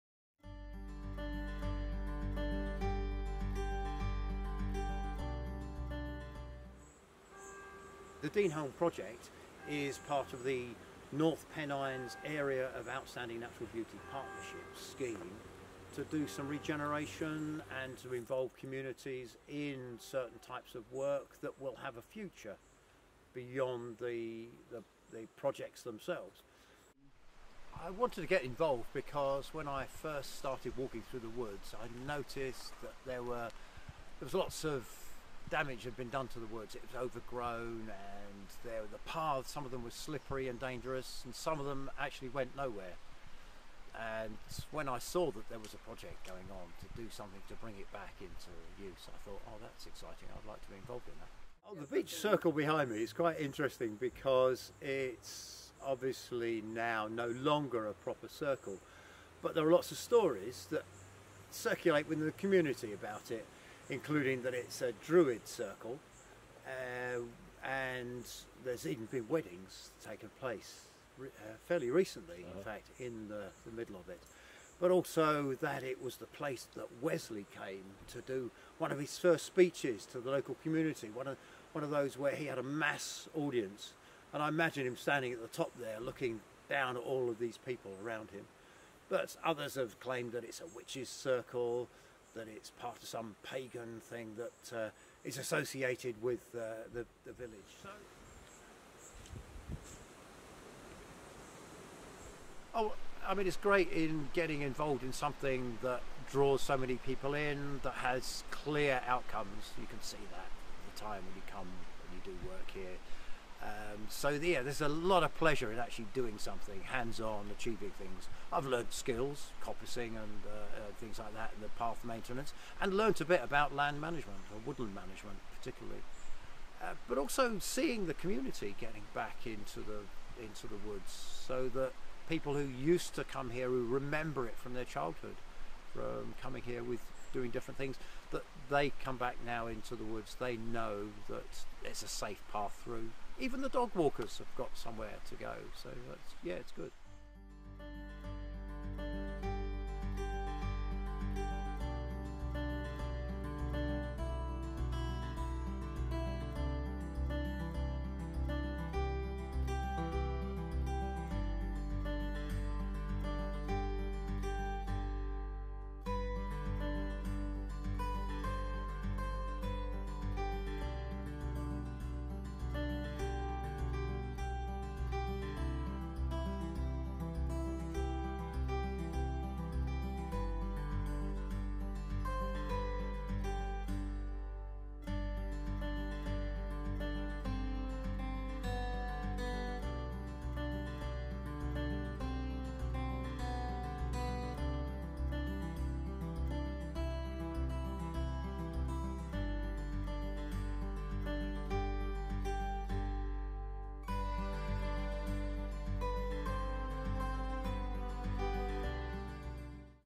Deneholme Woods, Allendale. Interview with Paul Mingard
An interview with Paul Mingard, Deneholme Wood Volunteer.